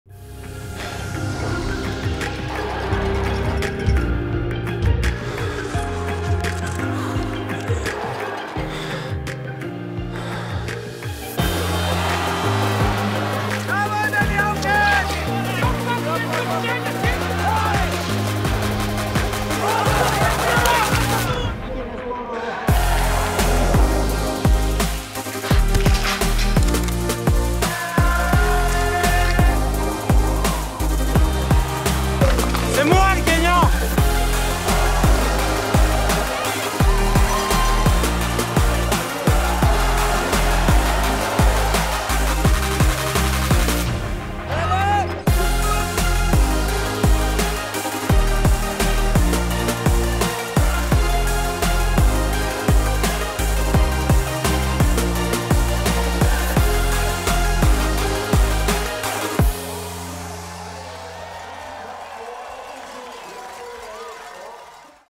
BMW IBU World Cup 2022-2023 Season Trailer
Get ready for the return of great biathlon: the BMW IBU World Cup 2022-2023 kicks off on 29th November 2022 in Kontiolahti.

Download the official IBU app and follow your favourite athletes and venues to never miss a shot!

Who else is excited?